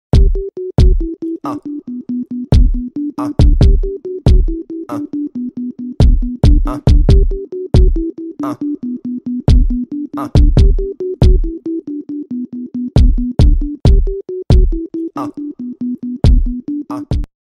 Snake Eats Mouse
articulated puppet animation... with apologies to Kid Cudi